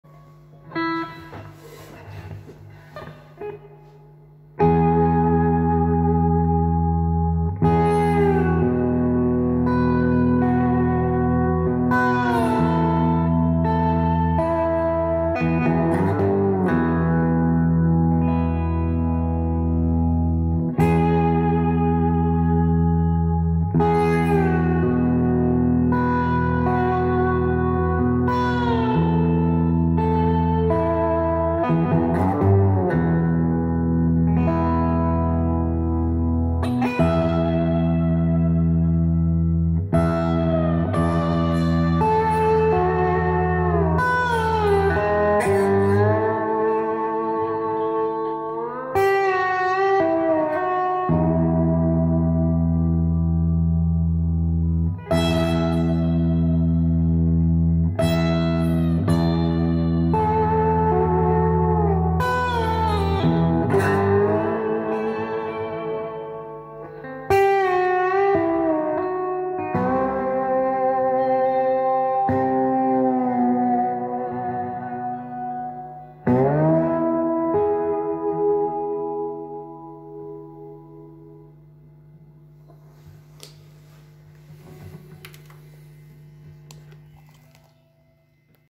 Gibson Flying V with Bigsby and TV Jones PU - Slideguitar Blues in Open E
Slideguitar Blues in open E-tuning